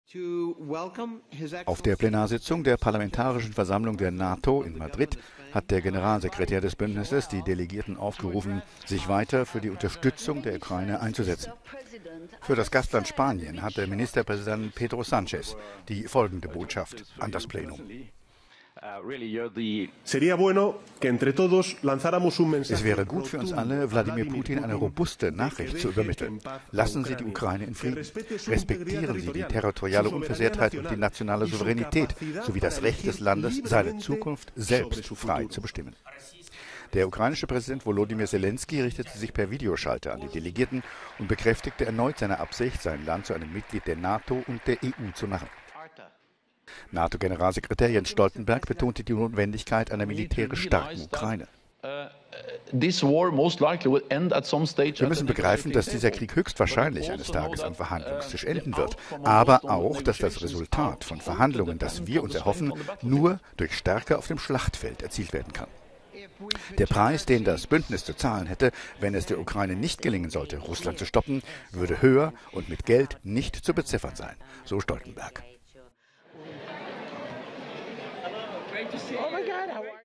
Parlamentarische Versammlung der NATO tagt in Madrid
Sanchez, Zelensky und Stoltenberg richten Appelle an die Delegierten der Bündnispartner